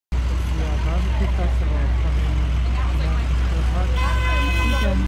NYC Madison Square Park BBQ festival
It’s the yearly BBQ fest!  Pits from all over the country have come to offer their own unique rubs and sauces!